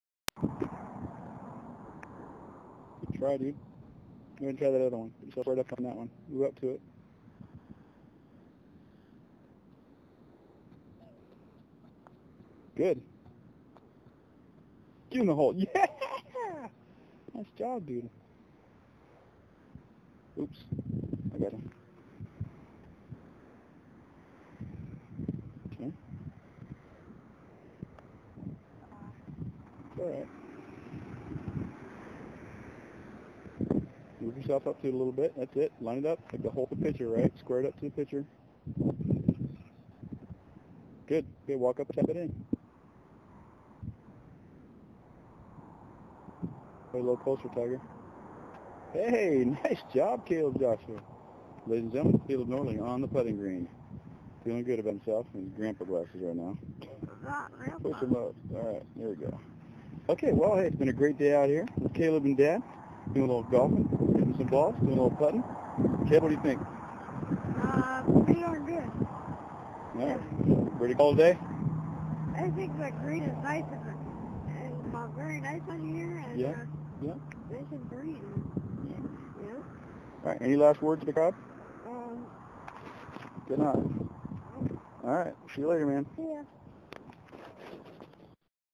Caleb and Golf 2
Here's Caleb on the putting green, all by himself knocking down a couple of putts...ATTA BOY!